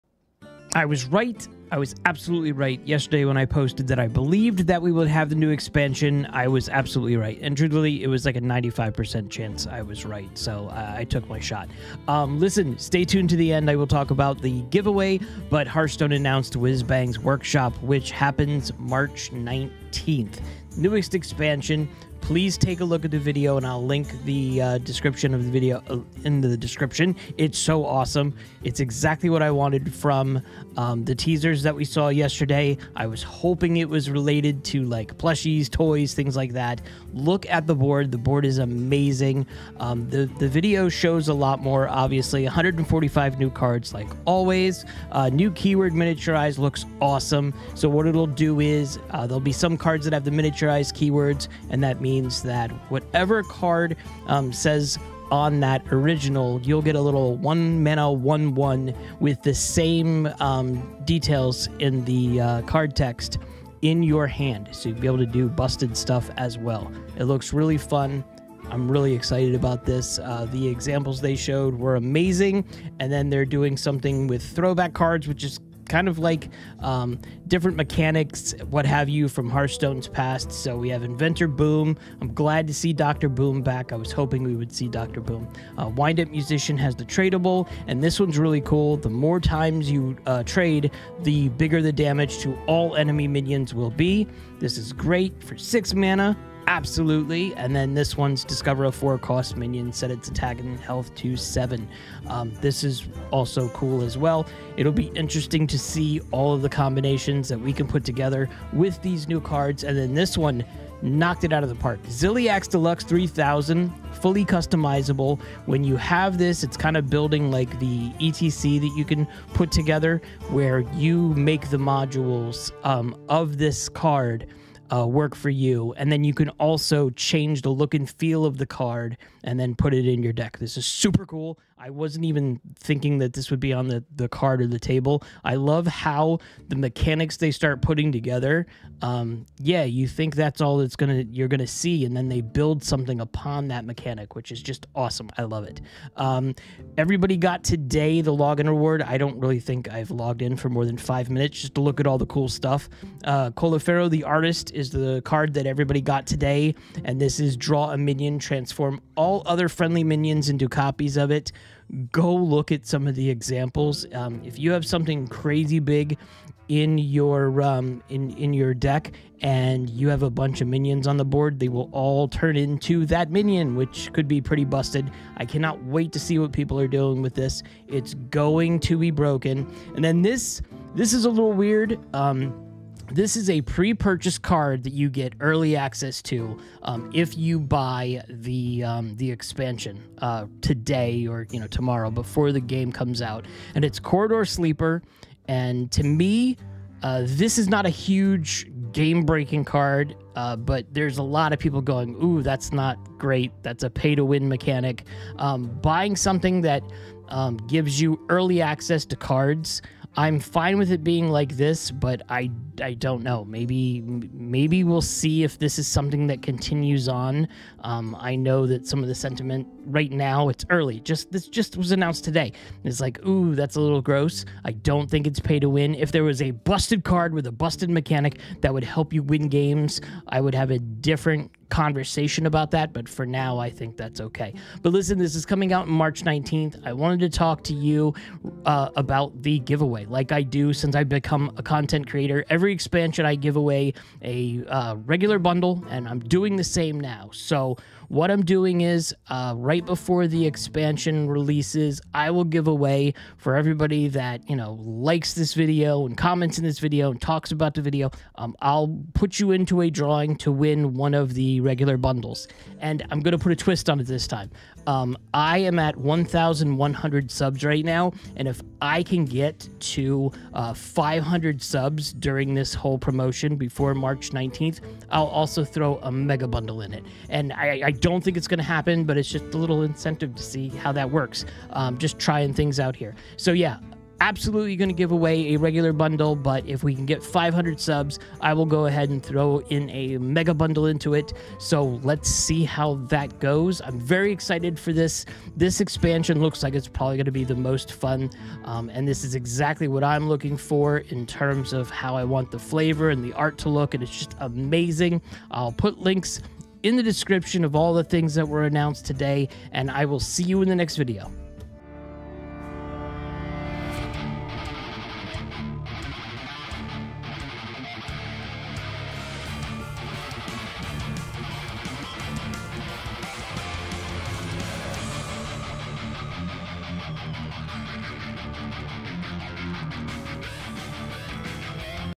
New Hearthstone Expansion Details and BUNDLE GIVEAWAY! Whizbang’s Workshop!
Here it is, the new Hearthstone Expansion: Whizbang’s Workshop!

The genius inventor and toymaker, Whizbang, has opened the doors to his magical workshop—and all of Azeroth’s invited! This place is teeming with plushies, Miniatures, and more. With nonstop nostalgia and options galore, you’re bound to find a new favorite toy—or even build one yourself. Welcome to a world of whimsy and wonder, welcome to Whizbang’s Workshop!